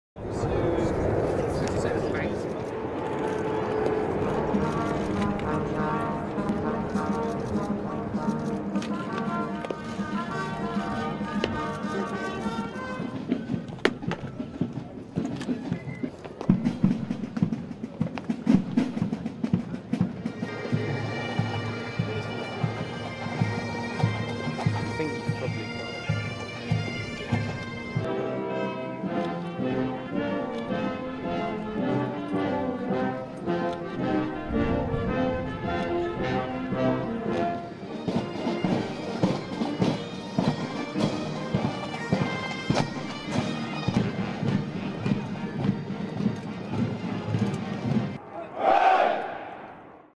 Prince William and Kate celebrate St Patrick's Day
Britain's Prince William and his wife Kate, the Duchess of Cambridge, honoured the 1st Battalion of the Irish Guards on Ireland's national day, St Patrick's Day (March 17), handing out shamrocks to the soldiers and the regimental mascot - an Irish wolfhound.